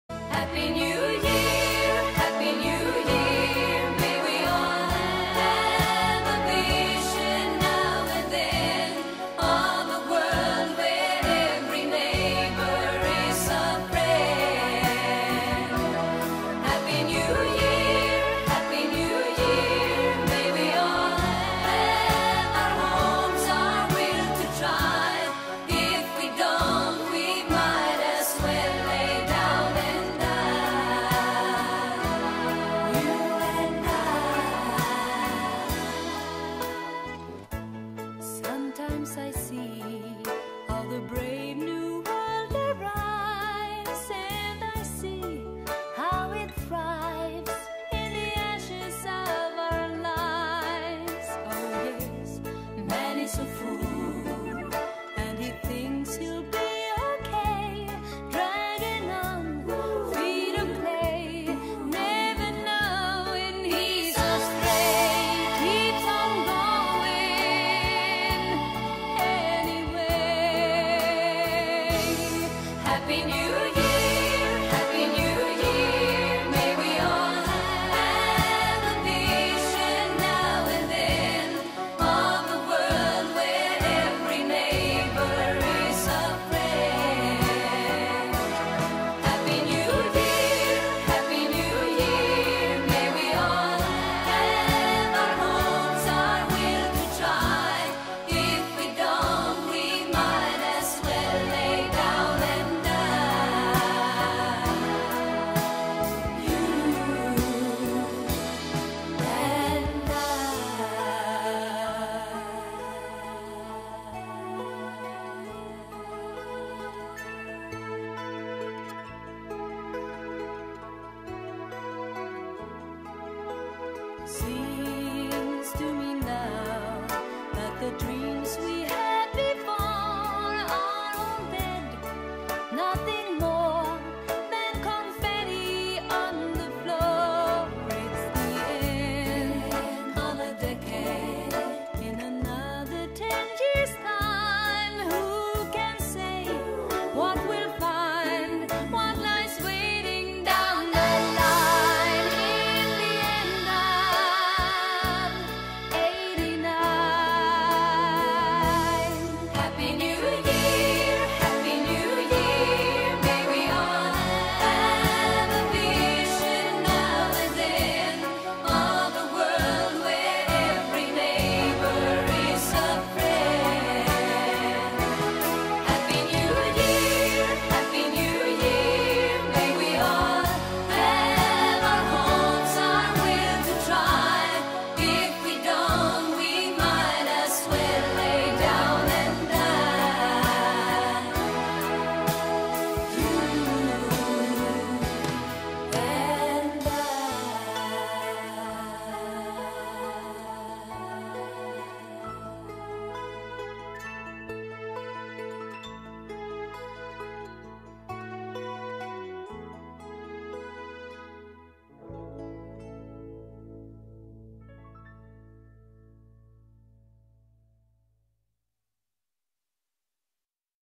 Fireworks filmed with drone in Telenesti - Moldova 31.12.2017
Drone: Phantom 3 Standard